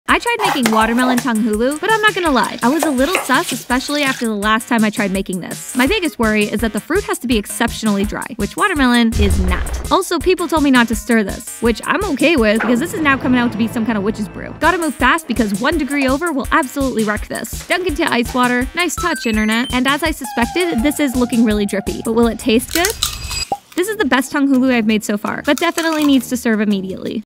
My second Tanghulu attempt turned into a drippy watermelon fiasco!
So... my second Tanghulu attempt turned into a drippy watermelon fiasco!

✨recipe:
✨skewer fruit on a stick (watermelon, strawberries, grapes, etc.)
✨in a small saucepan, heat up 2 cups of sugar and 1 cup water over high heat. Boil and with a thermometer make sure the mixture gets to 300F (no less). DO NOT STIR. Once it gets to 300F, remove from heat immediately, and start rotating the skewers into the sugar mixture.
✨Best practice is to immediately remove the fruit out of the sugar mixture and plunge into ice cold water. Serve immediately and enjoy 😉